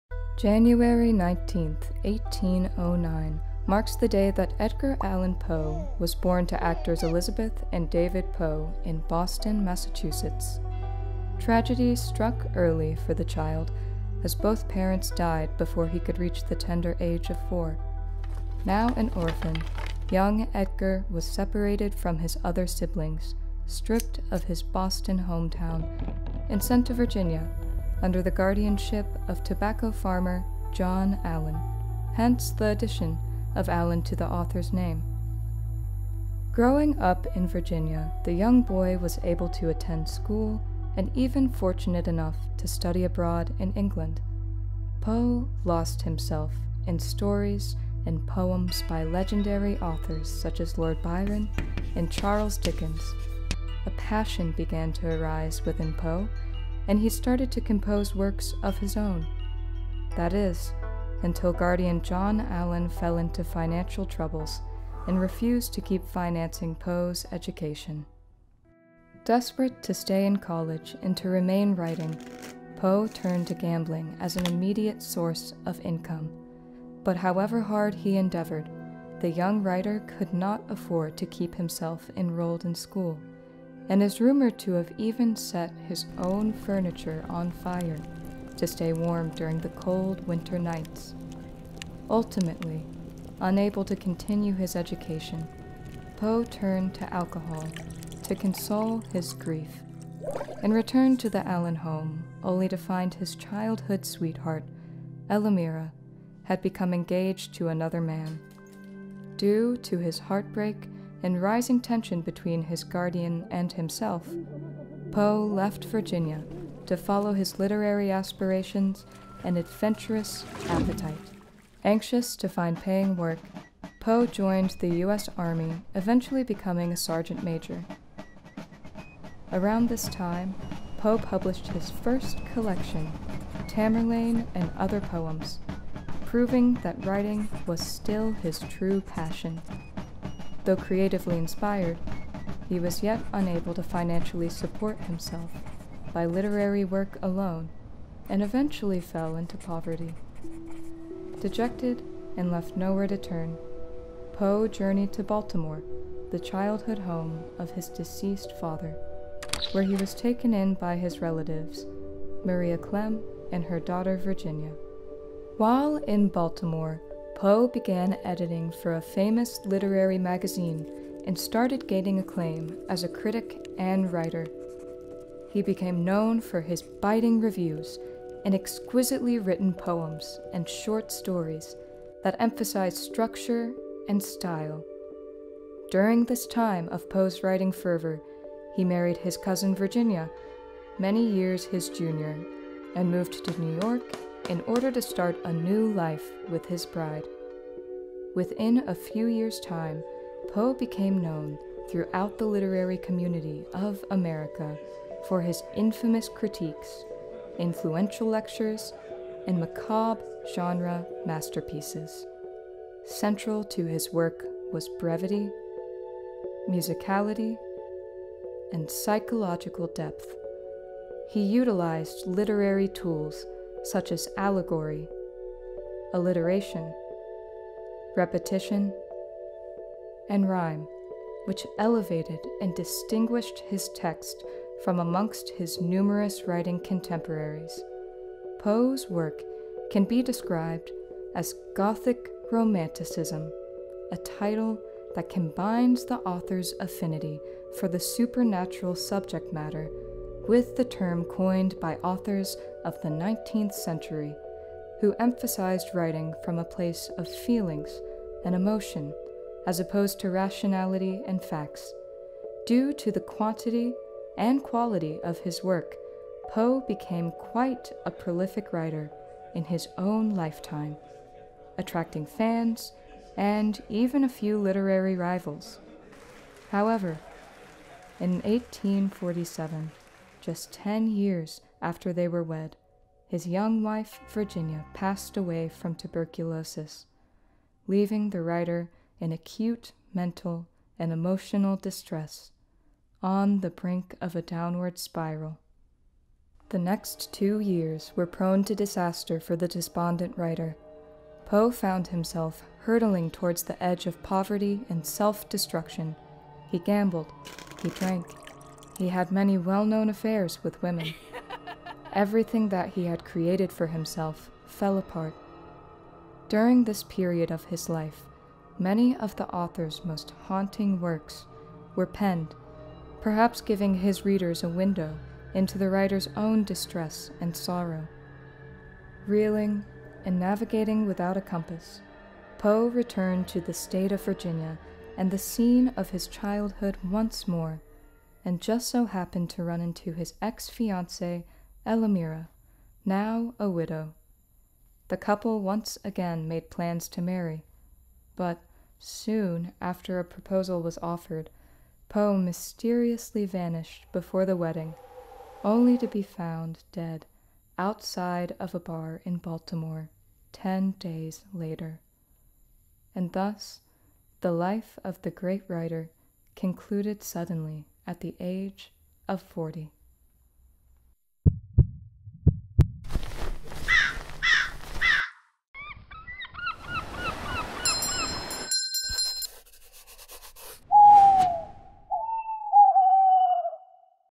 An Animated Biography of Edgar Allan Poe
In a 19th century world saturated with author’s looking to make their mark upon the early American Literary scene, why is it that from among the thousands of texts produced, Edgar Allan Poe’s works remain cemented at the core of the American canon?... Poe was a master of language, structure, and genre. He wrote with a command over his texts and an understanding of narrative and diction that have been endlessly emulated by numerous other artists throughout the decades. Therefore, the question must be proposed, Who was Edgar Allan Poe? Upon investigation, it seems as if the man himself was just as much of a mystery as the poems and stories he penned to paper.  How is it that a man oft regarded as a gambler and a drunk who mysteriously died without a penny to his name, lived on in infamy through the pages of some works that he never even lived to see published? Well, in order to identify the answer to these puzzling questions, one must start like any other good story, at the beginning.

Illustrated and Animated by Justin Dormitzer - ig: @justdorm
Written and Directed by Ashlyn Frank - ig: @ash.grace.frank
Voiced by Emma Payne